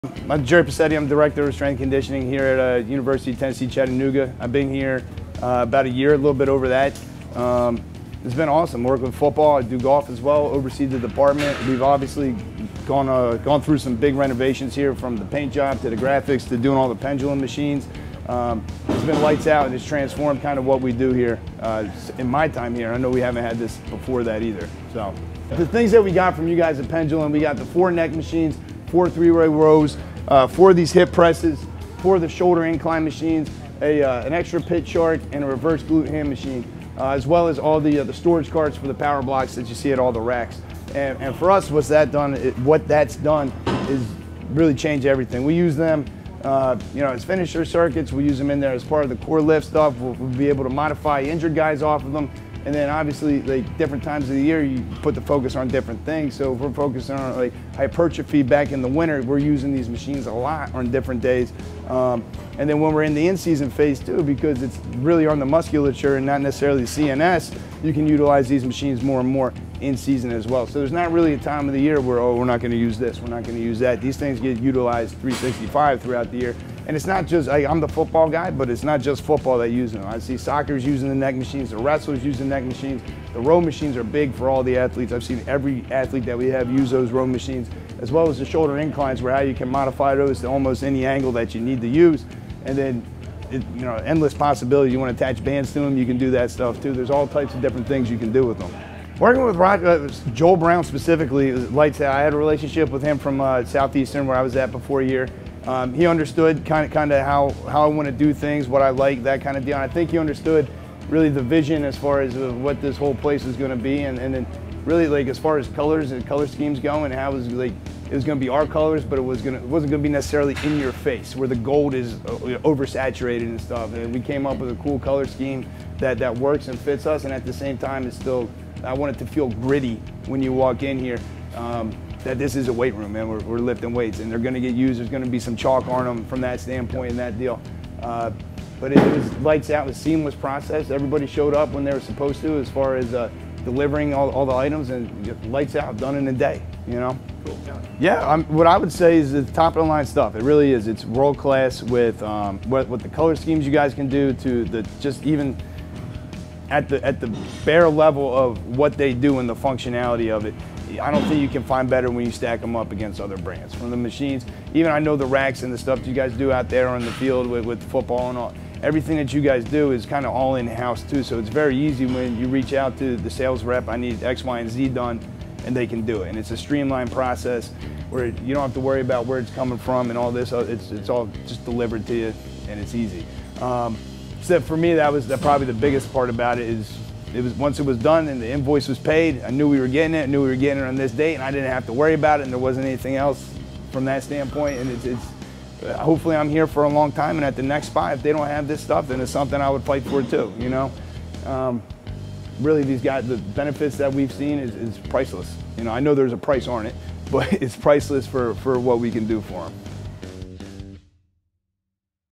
The University of Tennessee at Chattanooga Weight Room
The Chattanooga Mocs recently added Pendulum Strength Machines as part of their weight room renovation. Hear from Gerry Pacitti, Director of Strength and Conditioning, about how they train, his history with Pendulum, and how these machines have been a game-changer for their program.